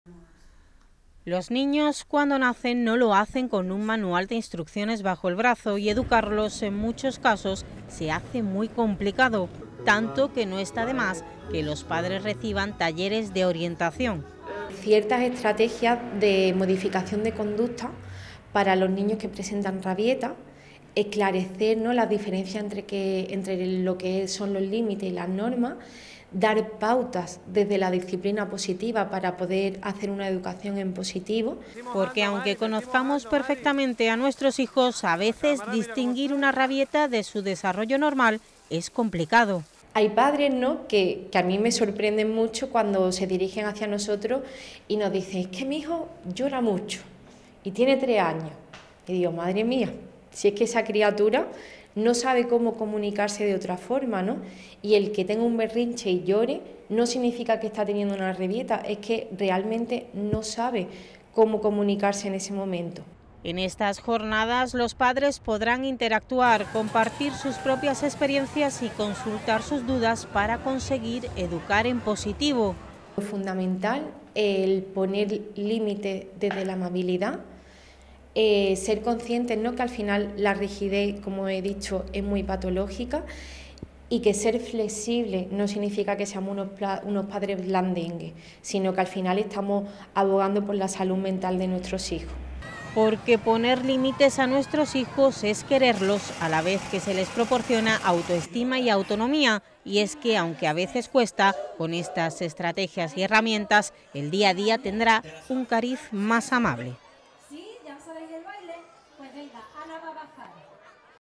Educar en positivo es fácil si se sabe como, cursos para aprender a ser "más amables"
Educar en positivo no es difícil si se sabe como hacerlo. Y, para ello, para adquirir los conocimientos necesarios, la psicóloga Isabel Morales imparte esta semana un taller específico en la Estación de Ferrocarril. Ha empezado este lunes y finaliza el jueves, de cuatro a siete de la tarde.